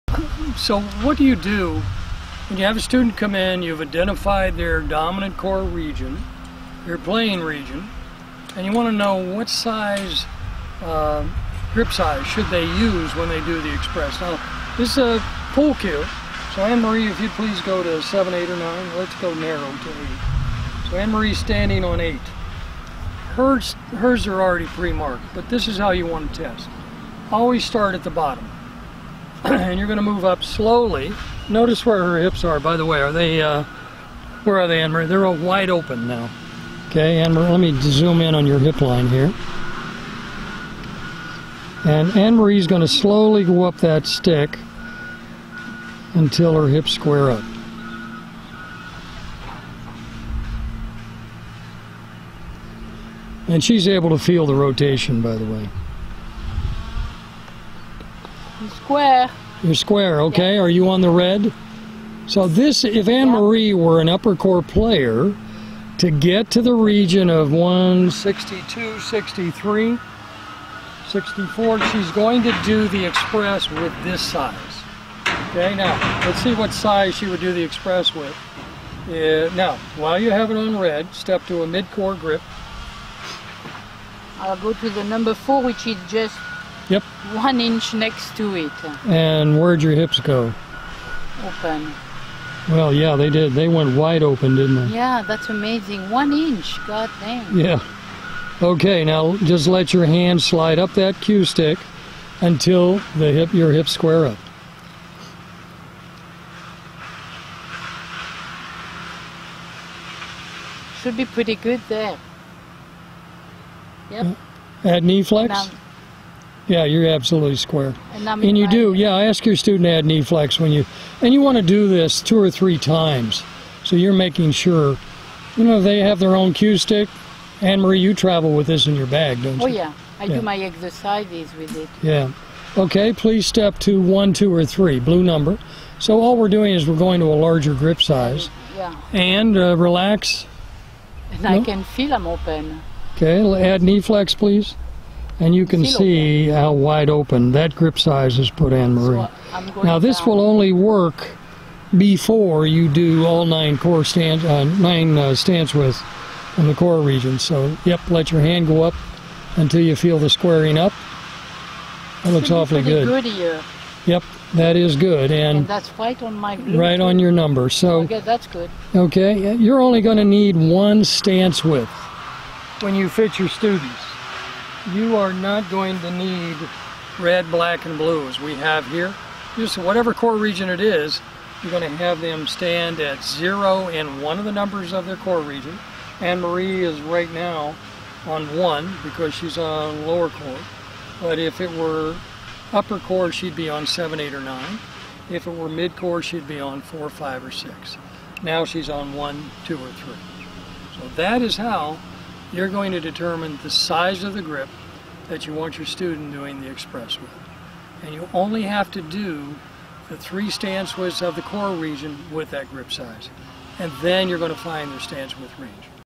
Finding Grip Size by Core Region
Dr. Wright and LPGA Veteran Anne Marie Palli demonstrate how to determine grip size using a pool cue stick and the 9 Stance Widths of Wright Balance.  As Anne Marie lets her hand move up from the tapered end of a cue stick you will see a rotation of her pelvis to square when she finds the correct grip size for whatever Core Region matches her stance.